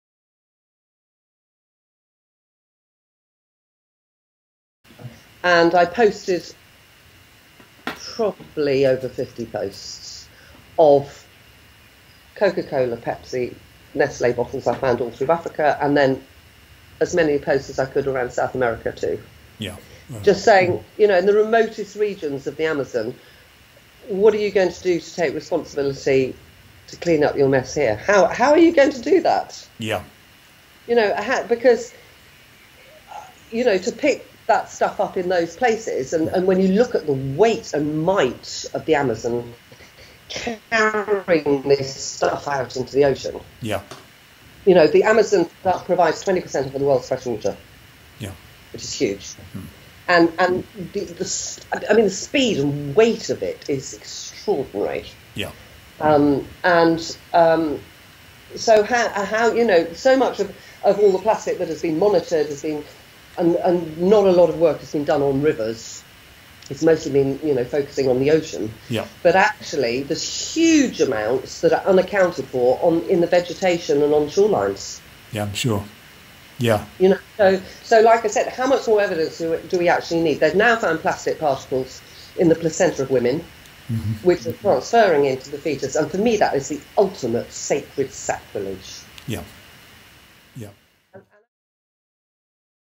Melinda Watson, Corporate Responsibility for Plastic pollution.
FB: Raw Foundation and Instagram : Rawfoundationuk
All South America photos : Elizabeth Streeter. @elizabethstreeterphoto

RAW TRUTH: PLASTIC
A JOURNEY FROM SOURCE TO SOLUTION